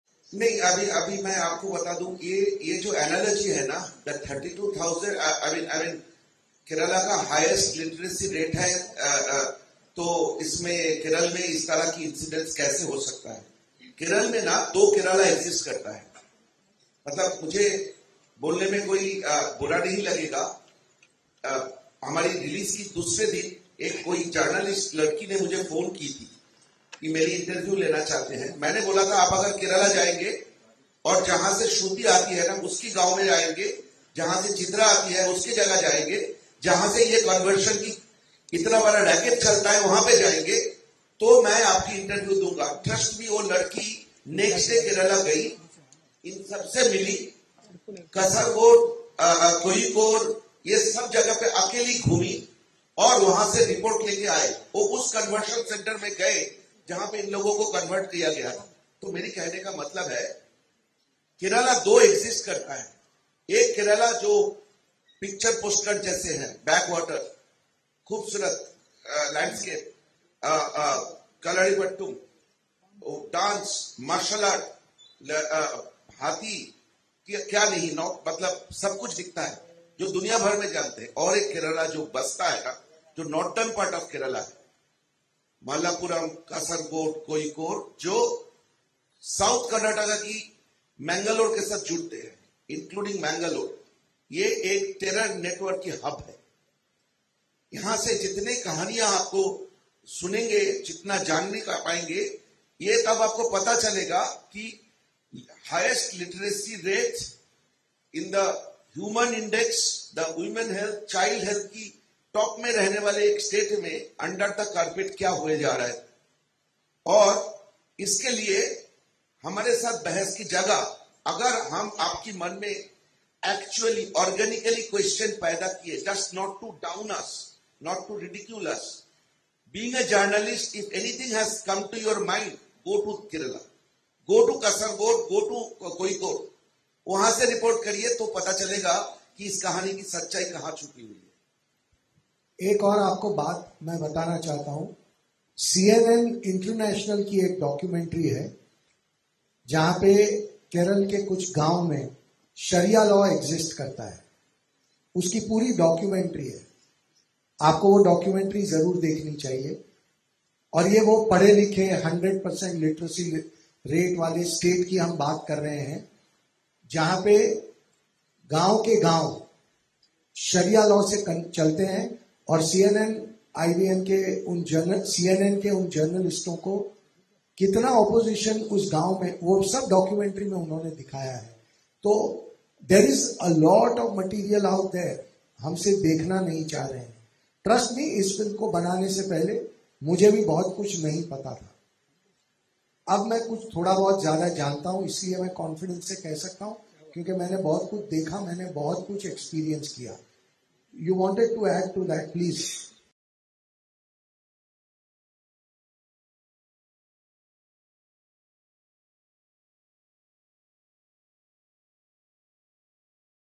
The Kerala Story: 'केरळमधील एकच भाग फक्त लोकांना माहीत आहे...'; केरला स्टोरीच्या निर्मात्यांचा दावा
The Kerala Story: ‘द केरला स्टोरी’ या चित्रपटाने बॉक्स ऑफिसवर रेकॉर्ड ब्रेक कमाई केली आहे. मात्र, अनेक ठिकाणी या चित्रपटावरून वाद निर्माण झाला आहे. अनेकांनी या चित्रपटाला प्रोपगंडा चित्रपटही म्हटले आहे. यावर उत्तर देताना केरला स्टोरीचे निर्माते आणि दिग्दर्शकांनी अनेक प्रश्नांना उत्तरं दिली. केरळमधील साक्षरता एवढी असताना तिथं असं कसं घडू शकतं? या प्रश्नाला उत्तर देताना 'केरळमधील एकच भाग फक्त लोकांना माहीत आहे. दुसऱ्या भागात या दहशतवादी गोष्टी सुरू होत्या' असं स्पष्टीकरण दिलं.